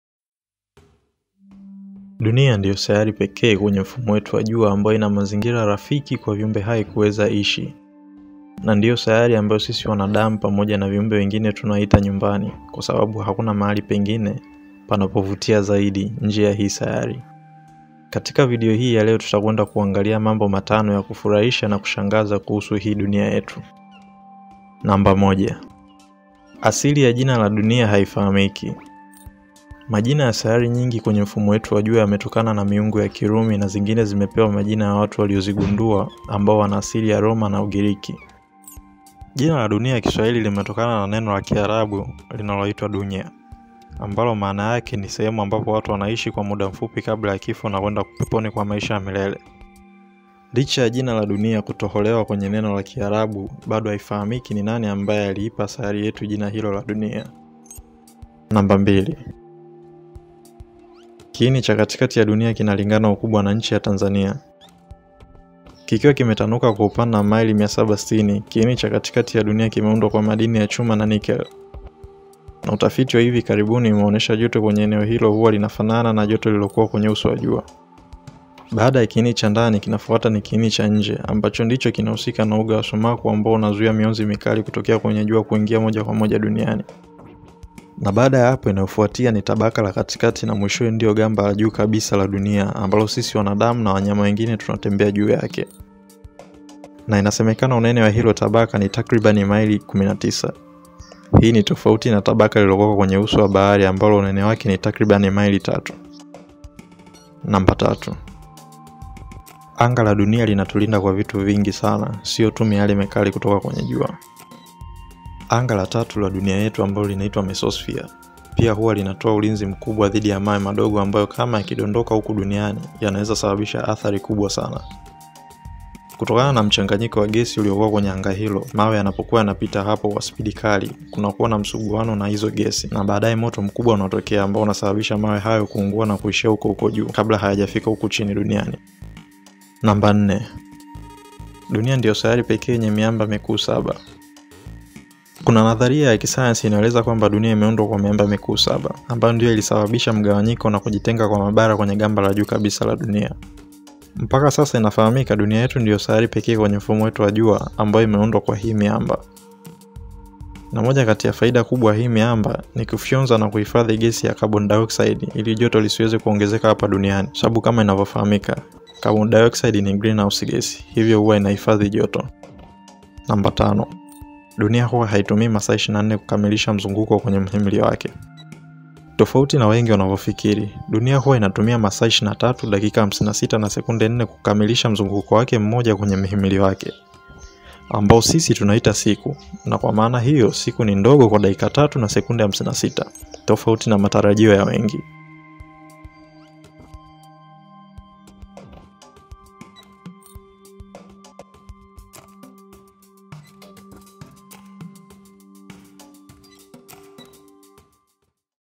Mambo Matano 5 Usiyofahamu Kuhusu Dunia yetu
Kama ukiitoa sayari ya Dunia basi hakuna sayari nyingine kwenye mfumo wetu wa Jua ambayo inaweza ikawa na mazingira rafiki kwa viumbe hai kuishi na hii ni moja kati ya sifa ambayo inaifanya sayari yetu ya Dunia kuwa ya kipekee sana. Katika video ya leo nimekuandalia mambo matano 5 usiyofahamu kuhusu Dunia yetu hii ambayo sisi tunaishi ndani yake.